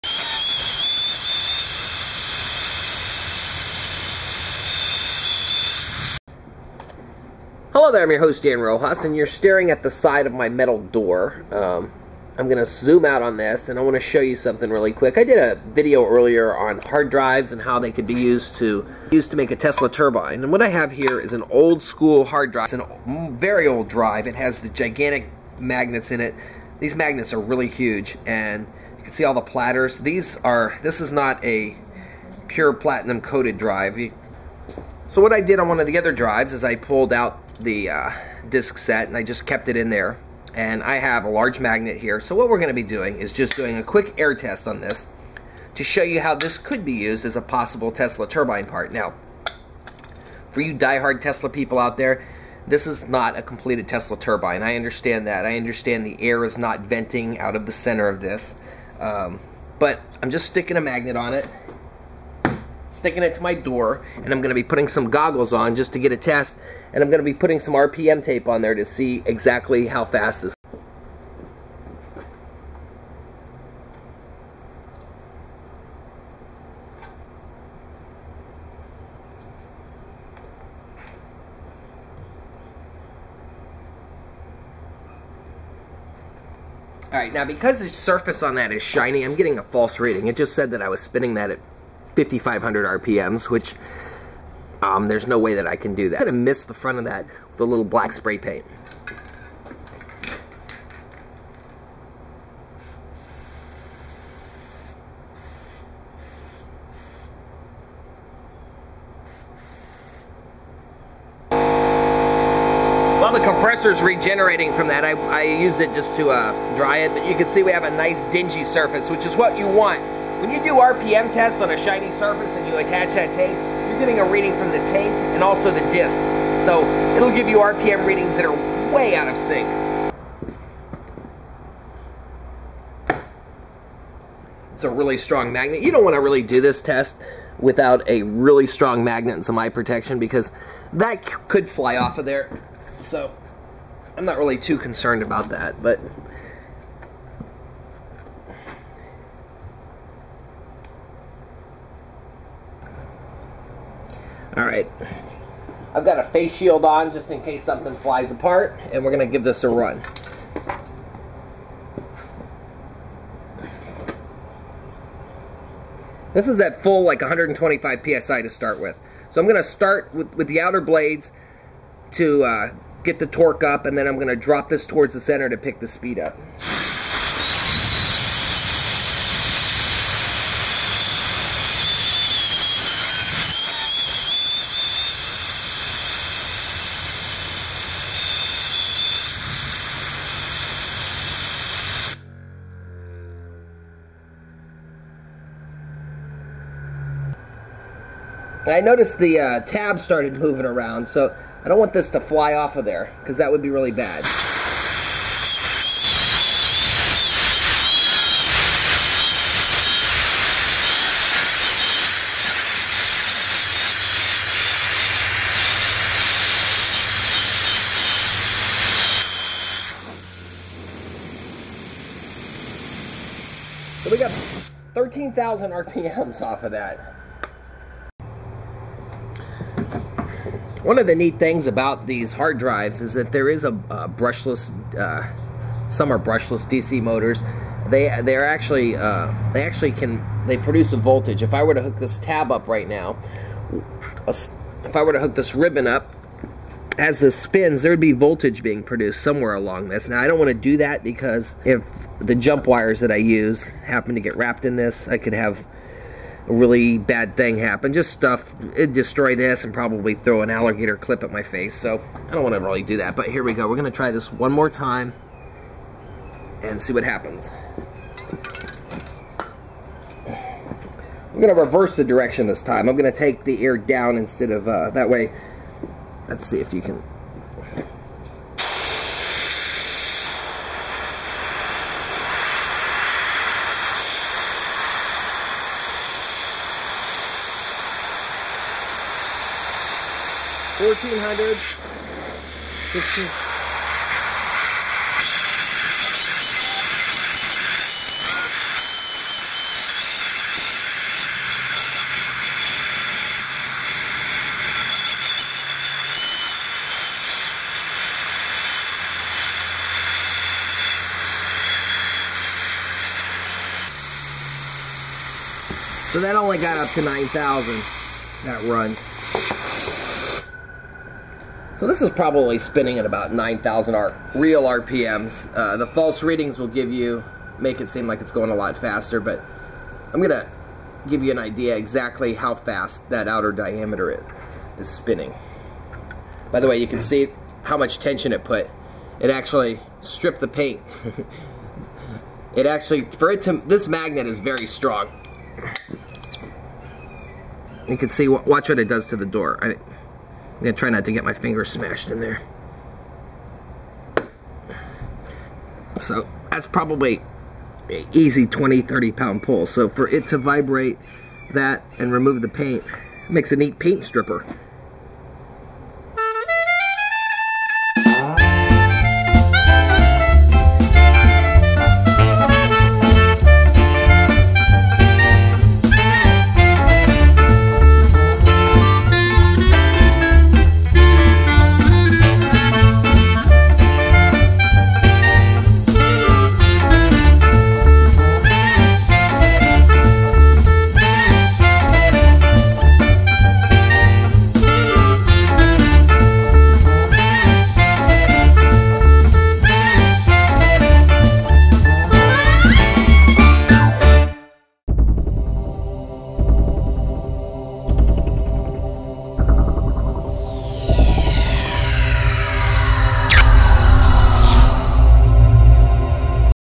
Tesla Turbine from Hard Drive Platters for electrical power generation energy free hack
This Tesla concept turbine option utilizes ten hard drive platters from a 1990 harddrive. It makes a great free spinning hack disk part for a Tesla Turbine. You would need to machine the centers for energy exhaust ports.